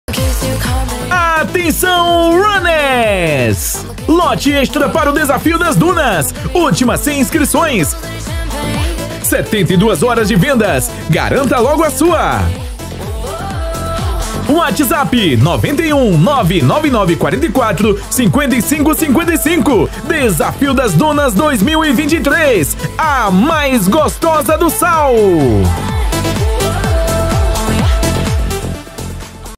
|| Aventura nas Dunas do Pará: Inscreva-se na Corrida mais Encantadora! ||
"Prepare-se para uma aventura emocionante no próximo domingo, 29 de outubro, com a corrida mais charmosa do estado do Pará: O Desafio das Dunas! 🏃‍♀️🏃‍♂️ Areias douradas e paisagens deslumbrantes esperam por você. 🏖️ Não perca tempo, garanta sua inscrição agora e junte-se a nós nessa jornada inesquecível. 🌅 DesafioDasDunas CorridaNoPará InscriçõesAbertas"